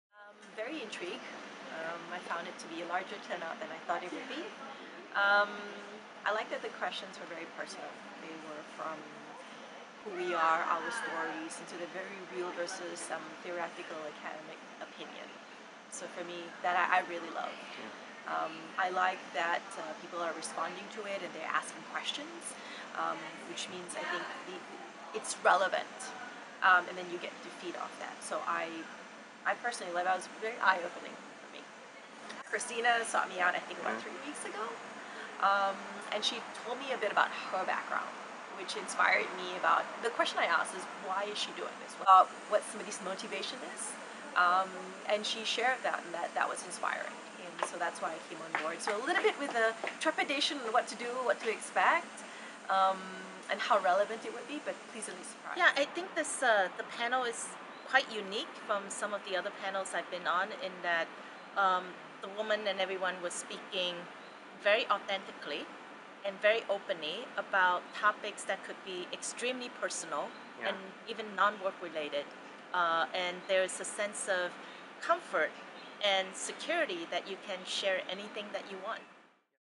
Jacqui and Cindy Testimonial
Jacqui Miranda of Microsoft and Cindy Deng of App Annie shared their thoughts on the recently concluded WomenChangemakers #11 event held in Singapore.